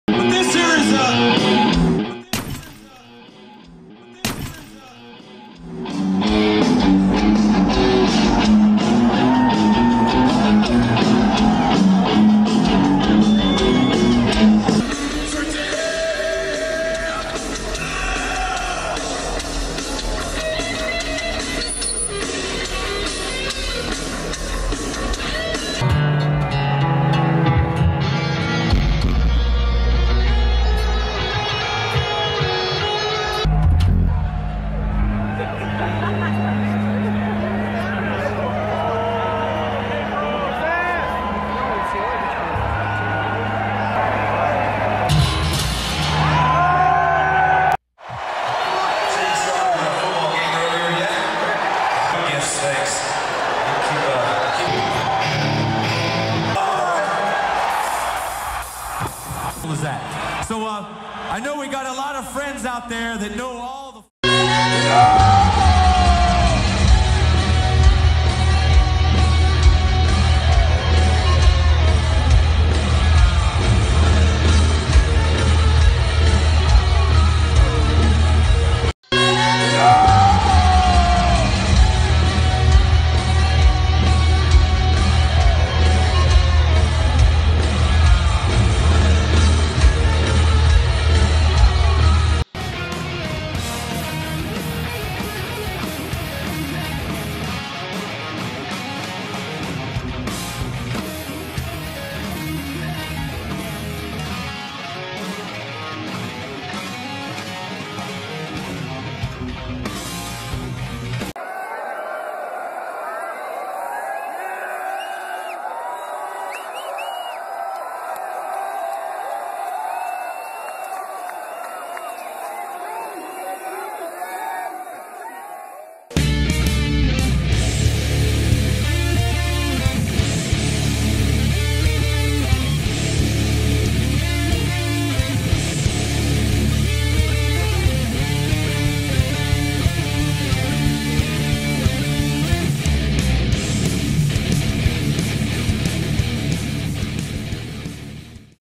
JAMES HETFIELD - BEST INCIDENTS | METALLICA
✔ James Hetfield -  Crowd Throws A Shoe, Mexico (2012)
✔ James Hetfield -  Newsted Pisses Off James (1992)
✔ James Hetfield -  Kicks Over Guitar, St.Louis (2017)
✔ James Hetfield -  Gets Pissed Off (2017)
✔ James Hetfield -  Fall Over, Amsterdam (2017)
✔ James Hetfield -  Joke To Security Guard
✔ James Hetfield -  Happy Birthday, Mexico  (2012)